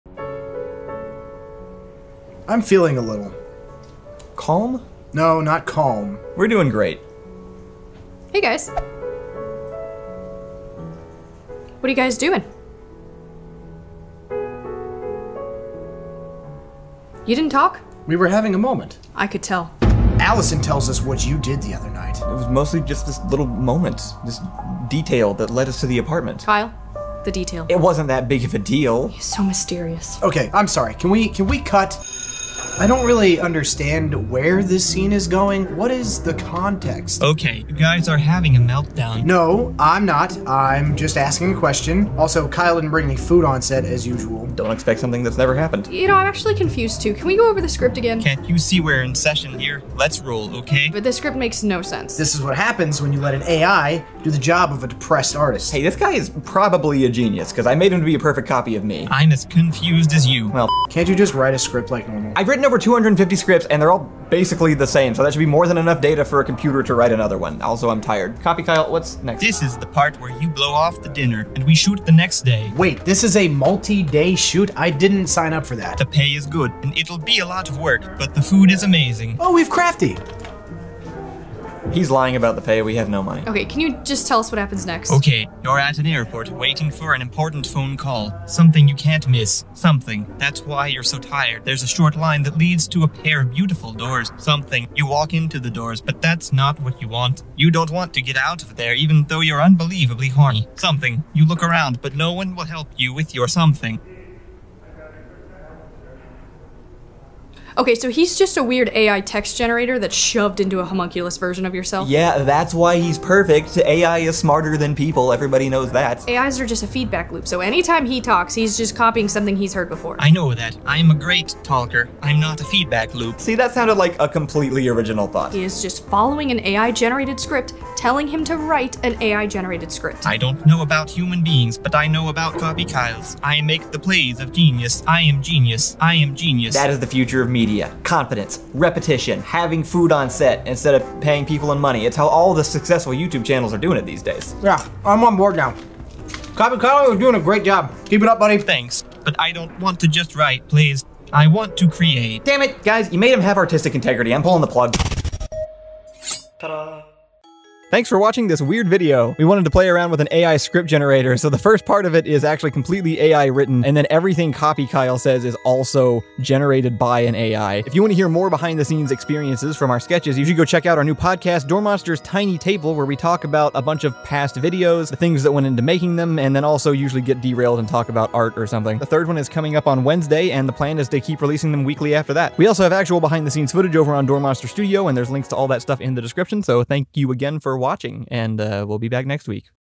AI Wrote This Comedy Sketch
We thought it would be fun to try and generate an entire Door Monster script with an AI script generator, but of course our comedy instincts took over and we had to start responding to it - the first "video-within-a-video" part is an AI script, and then we the humans took over writing the human lines, but continued generating them for CopyKyle.

Starring Kyle, Allison, and Thomas
with CopyKyle, CopyKyle, and CopyKyle